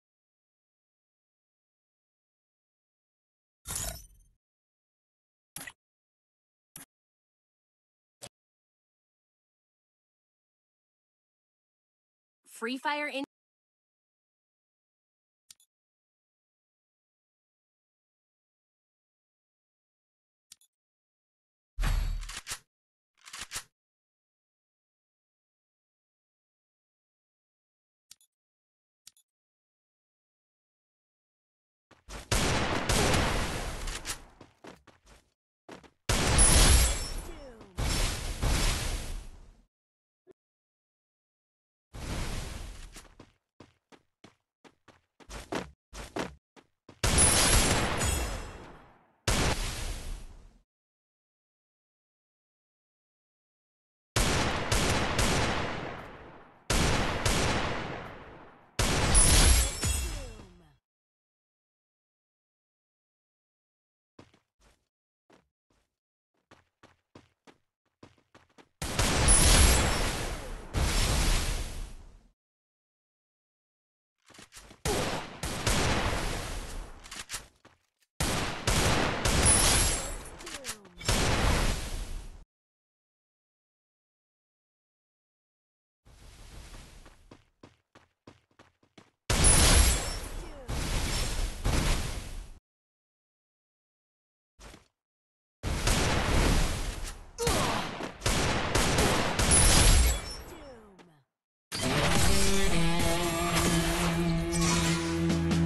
9 November 2024
My normal gameplay in custom.Me vs my friend.My id:SoumyaA1305A.